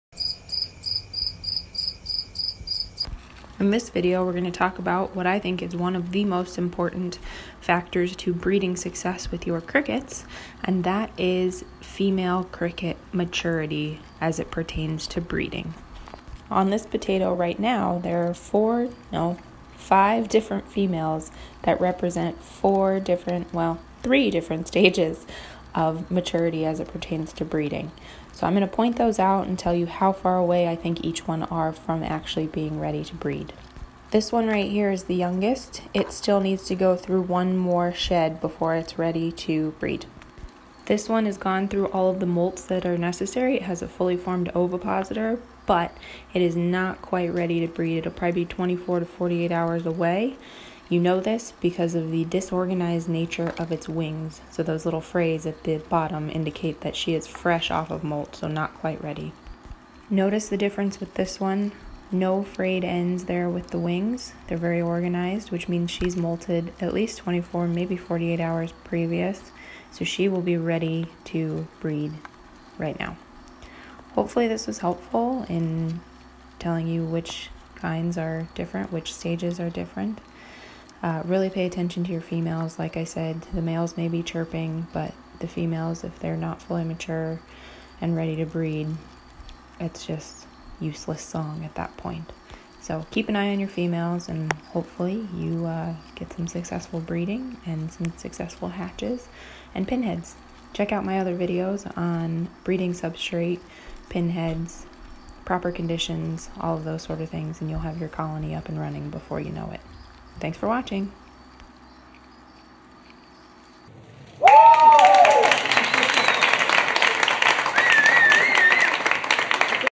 How to Breed Crickets: Female Cricket Maturity
In this short video, we cover some visual signs of female cricket maturity. There are 3 different stages represented.

1. The youngest is likely 2-4 days from being ready to breed
2. The middle is 24-48 hours from being ready to breed
3. The final example is ready to breed.

Hope this helps clarify what a female cricket ready to breed looks like.

It’s one of the single-most important factors for successfully breeding crickets.

Please drop any questions in the comments below.